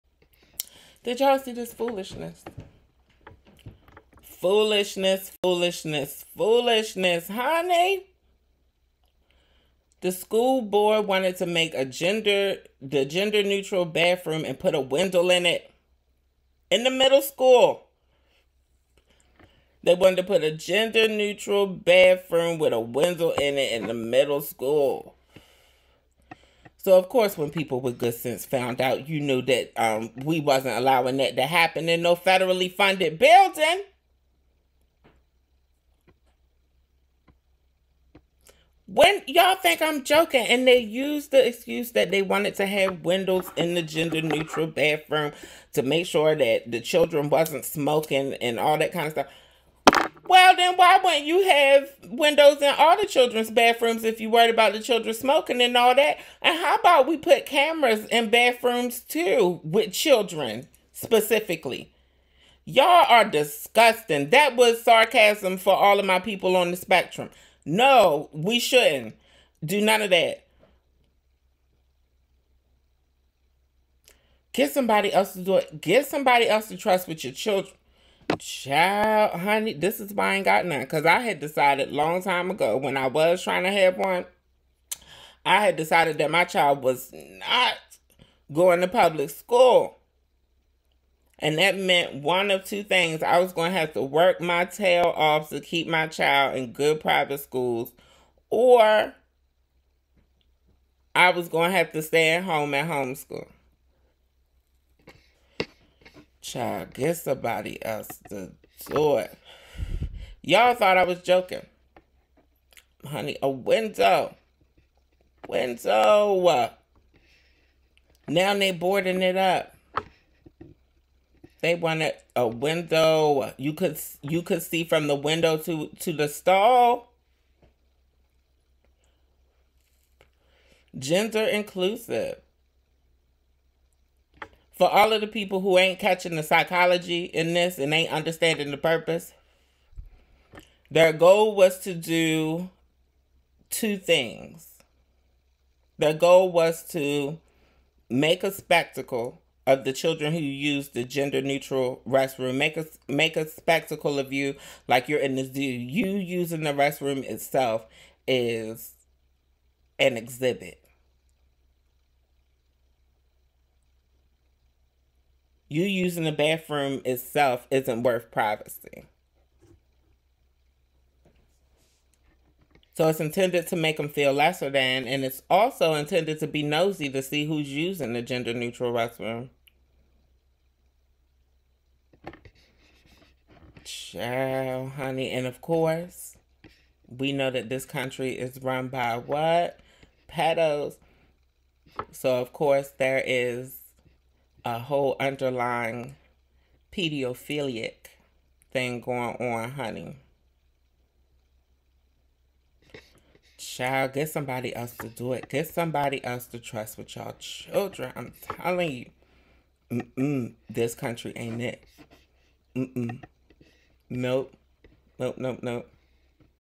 IKYFL!!?! Pennsylvania Middle School Puts Window in Bathroom to Expose Kids | Bre Starr | WWMW S3
Bre Starr (Fairy Puss) shares her perspective on current events in the LGBTQ and/or Black communities.

Ballroom Tea every Tuesday.

Wednesdays LIVE
LGBTQ+ News & Politics